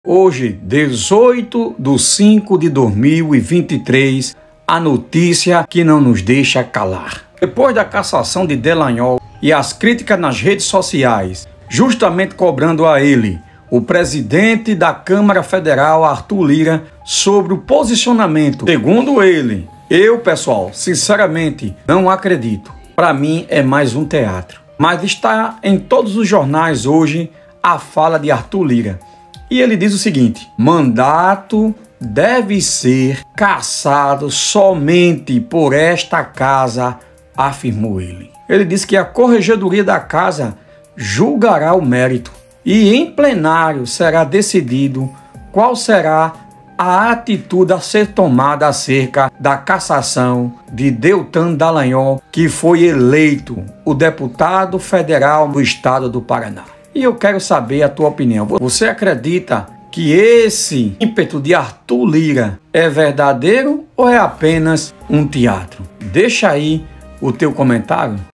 Arthur Lira diz, que cassação de um deputado, só tem legitimidade se julgado pela a Casa.
O presidente da Câmara dos Deputados, Arthur Lira (PP-AL), afirmou na noite desta quarta-feira (17/5), que a perda de mandato do deputado Deltan Dallagnol, decidida pelo Tribunal Superior Eleitoral (TSE), será analisada pela Corregedoria da Câmara dos Deputados. Segundo Lira, o ex-deputado poderá apresentar defesa.

Os procedimentos são regulamentados pelo Ato da Mesa 37/09. “A Mesa seguirá o que determina esse ato: a Câmara tem que ser citada, a Mesa informará ao corregedor, o corregedor vai dar um prazo ao deputado, o deputado faz sua defesa e sucessivamente”, disse Lira durante a sessão do Plenário. Lira afirmou, quando estava respondendo a uma questão de ordem do deputado Maurício Marcon (Pode-RS), que "o mandato deve ser cassado somente por esta Casa.